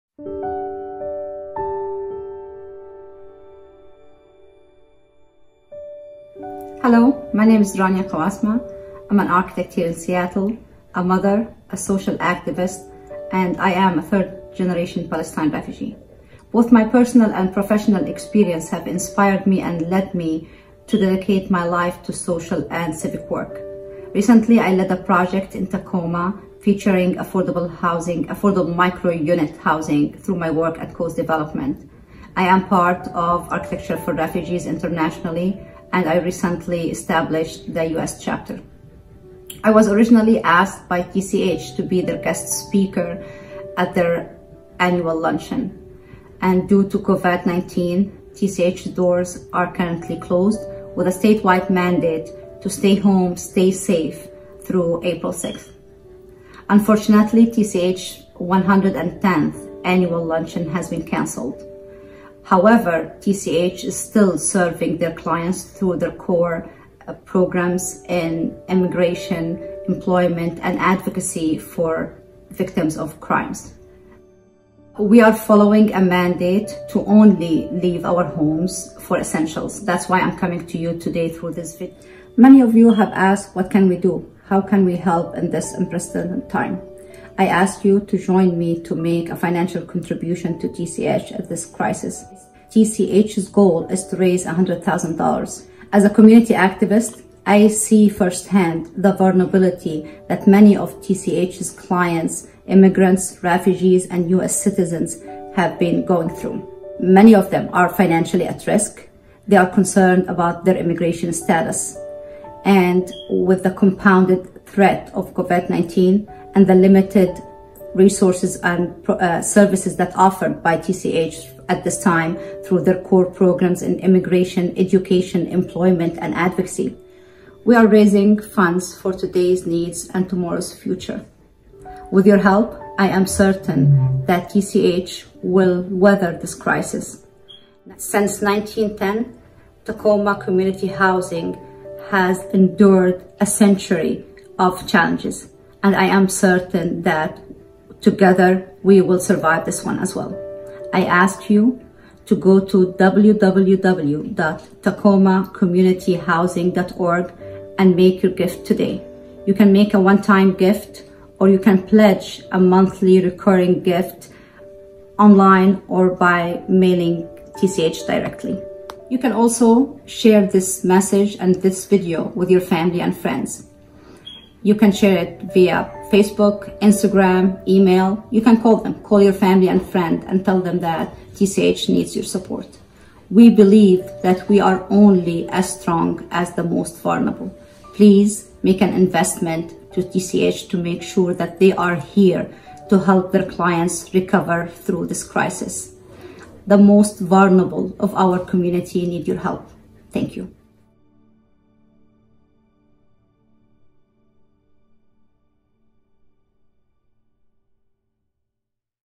Rania Qawasma-Dana | In Support of TCH
Rania Qawasma-Dana is a Palestinian-American Architect and public speaker who’s working in the areas of social equity, affordable housing and community-based design projects. She is a project manager at Koz Development, an affordable micro-housing company.

Rania works actively with local and international organizations tackling the displacement crisis and advocating for refugees. She’s part of the international Architecture for Refugees organization and founded Architecture for Refugees-USA with the vision to engage architects, designers, and community members to raise awareness of the issue of displacement.

One of the projects she created is “This Is Home,” an award-winning infographic guidebook to help newly arrived refugees to navigate through the neighborhoods they’re settling in. It’s designed to be a template to be used in several languages and different regions. The book is converted to an APP, which will be launched in early 2020.

Recently, she co-founded “Color my Home,” a community-based educational architectural design program that takes participants who faced displacement on a journey to trace, dream, and imagine their concepts of home.  Through her advocacy, Rania believes everybody deserves a place they call home.